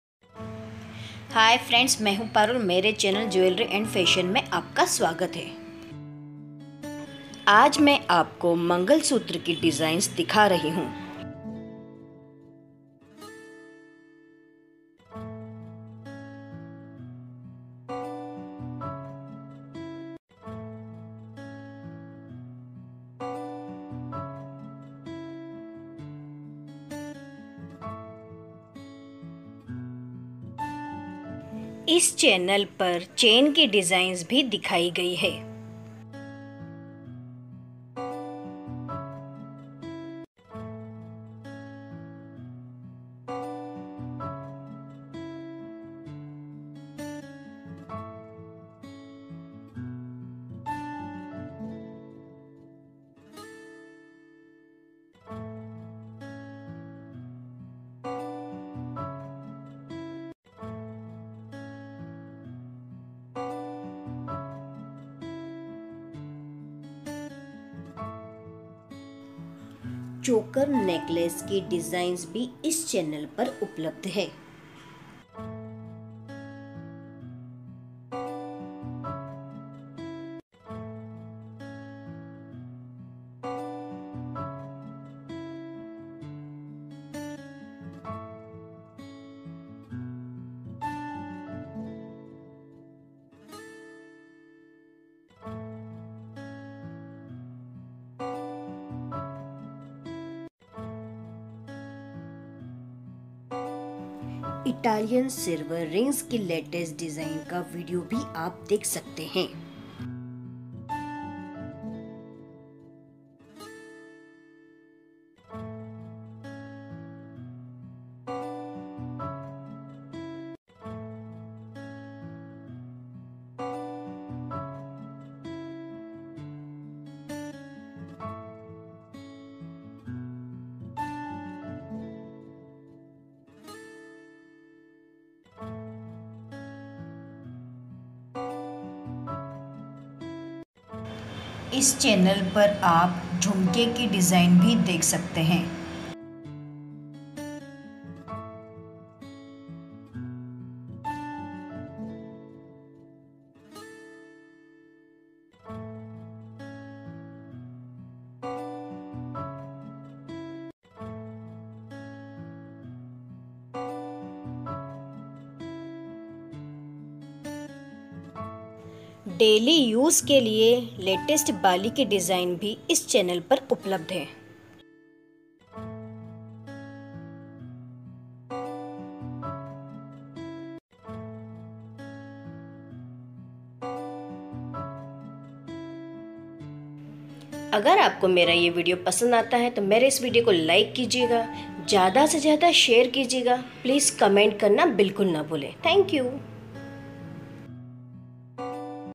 Daily wear mangalsutra designs in gold from 10 to 15 grams
Today we will match daily wear gold mangalsutra designs. Light weight gold mangalsutra designs from 10 to 15 grams.